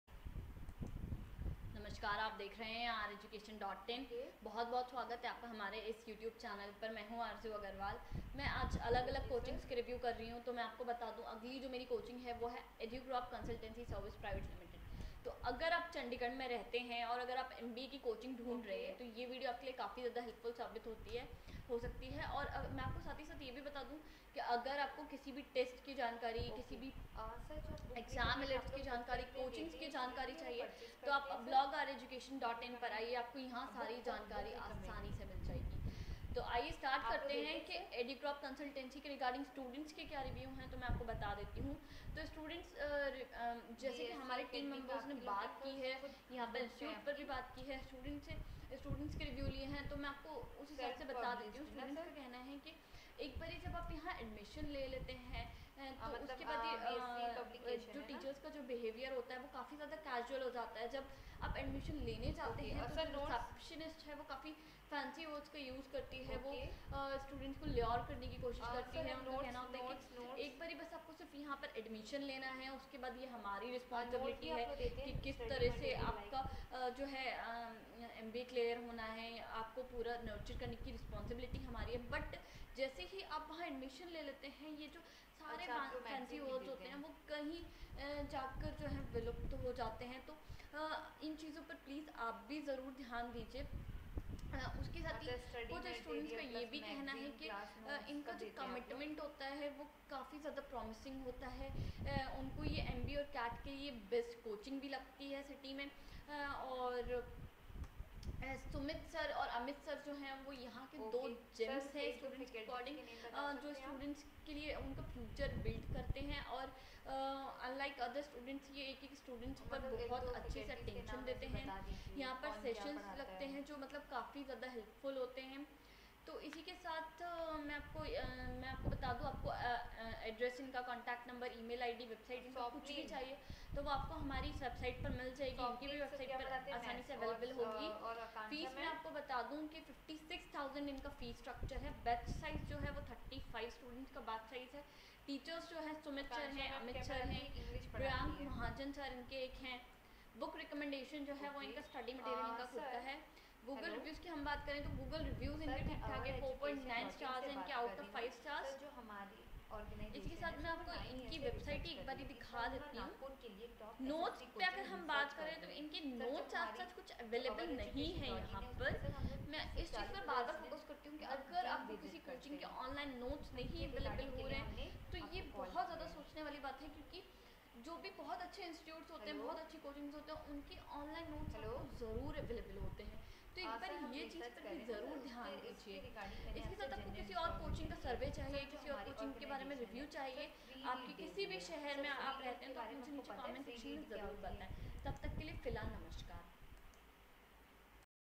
EDUCORP CONSULTANCY|| MBA COACHING IN CHANDIGARH| BEST MBA COACHING IN CHANDIGARH|| TOP MBA COACHING
This video is about the reviews of EDUCORP CONSULTANCY in Chandigarh. In this video, you will get the complete details about the coaching such as- infrastructure, students reviews, environment, etc. This video is very helpful for the candidates who are prparing for MBA and looking for MBA Coaching in Chandigarh.

You will get to know about behavior of teacher ,Conditions of campus, fees structure,books
video tells you the complete review of educrop consultancy coaching in chandigarh .  students looking for MBA coaching , this video can be very helpful for them .